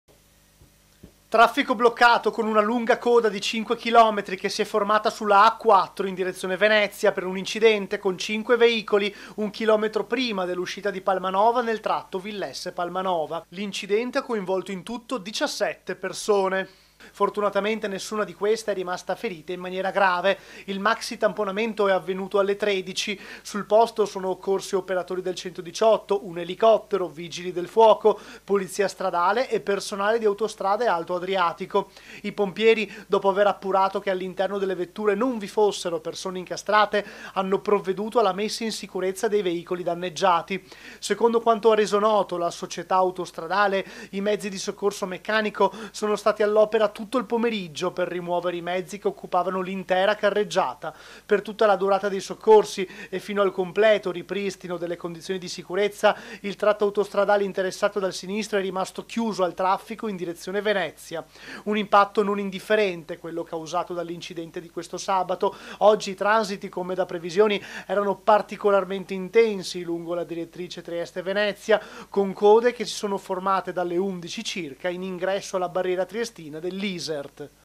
MAXI INCIDENTE IN A4: TRAFFICO BLOCCATO | 06/01/2024
A3 NEWS Treviso 06/01/2024 - TRIESTE - Maxi incidente in autostrada A4: traffico bloccato e lunghe code per tutto il pomeriggio. Nel sinistro sono rimaste coinvolte 17 persone. (Servizio di Gianluca Paladin) - Segui Antenna Tre anche sul digitale terrestre! Visita il sito